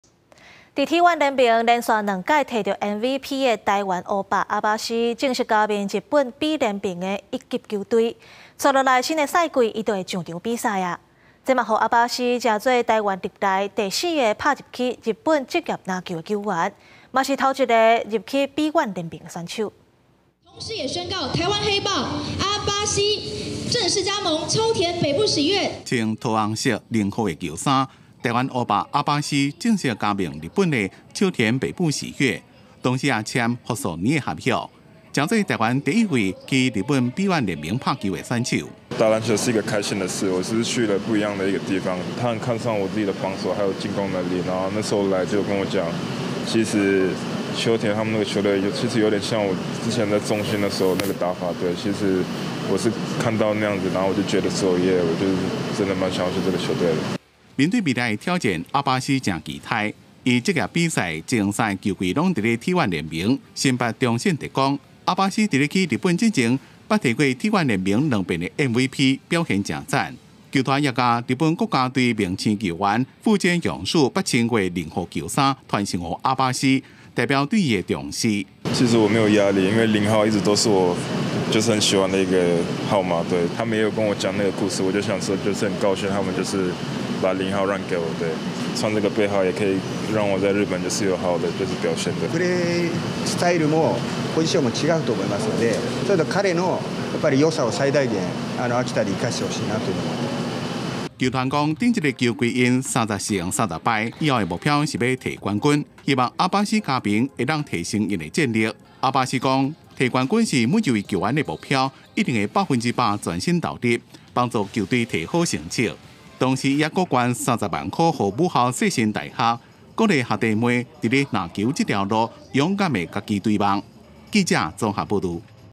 臺灣烏豹阿巴西 入日本職籃「秋田北部喜悅」 | 公視台語台 | 20240617
在T1聯盟，蟬聯兩度MVP的台灣黑豹阿巴西，正式加盟日本B聯盟一級球隊，將會在新賽季，披上「秋田北部喜悅」的戰袍出賽。這也讓阿巴西，成為台灣史上第四位打進日本職籃的球員，更是第一位進軍B1聯盟的選手。

四節新聞 ：　逐工（每天）09:00 / 12:00 / 15:00（拜一至拜五） / 19:00